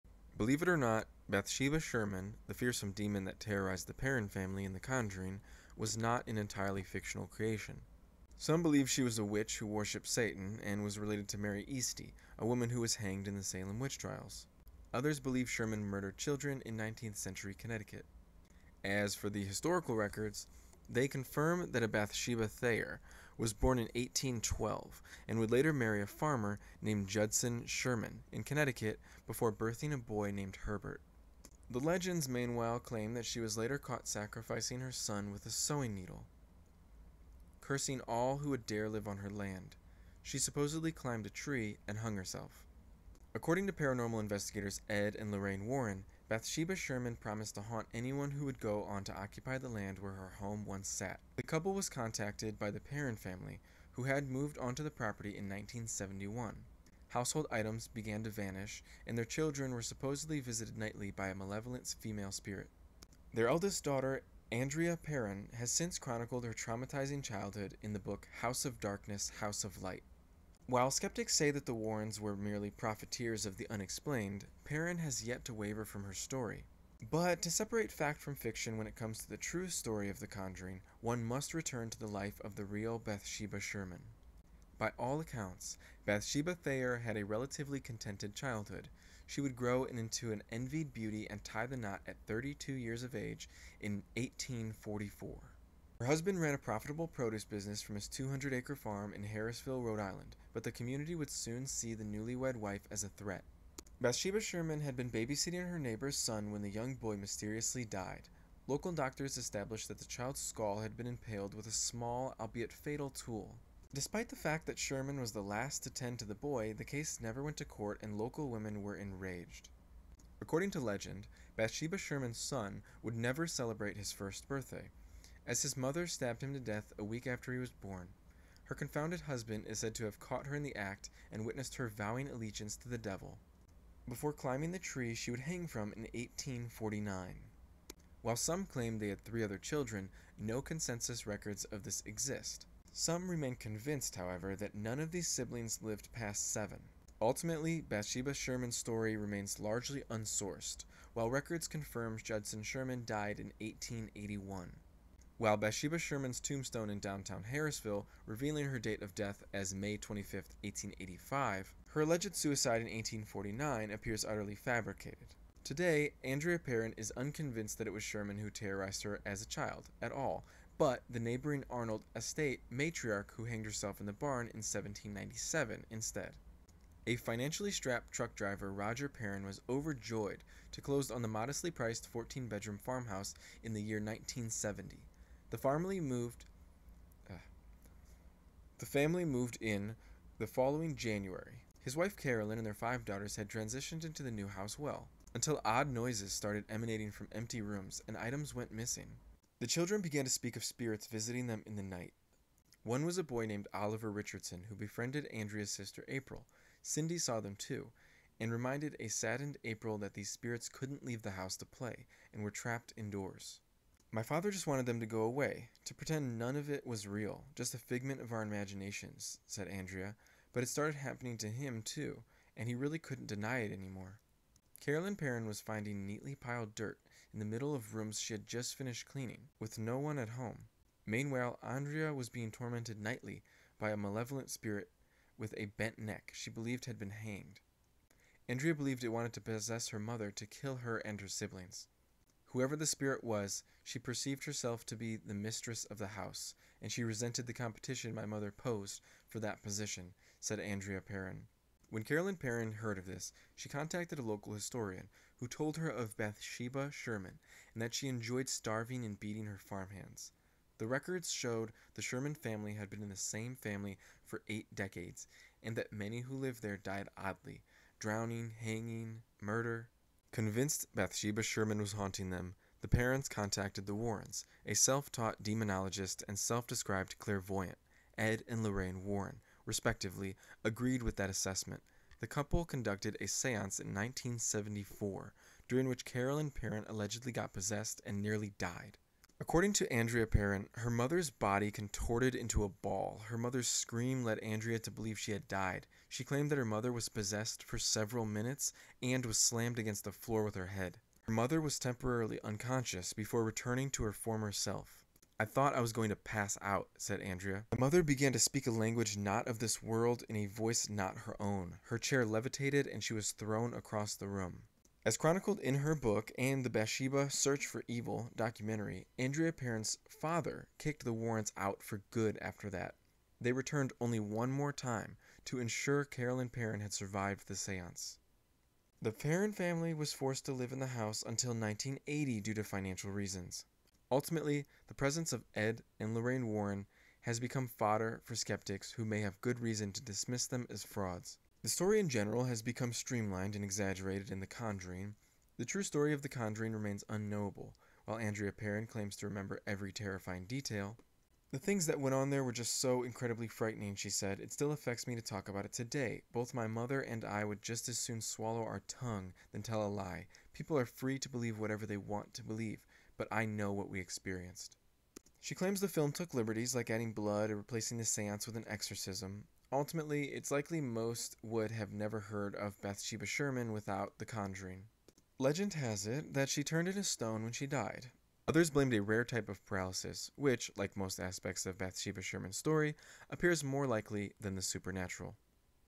Bathsheba Sherman, the Chilling Specter of 'The Conjuring' Saga
Explore the haunting saga of Bathsheba Sherman, the real-life ghost behind 'The Conjuring,' revealing the spine-chilling truth and untold mysteries surrounding her malevolent presence.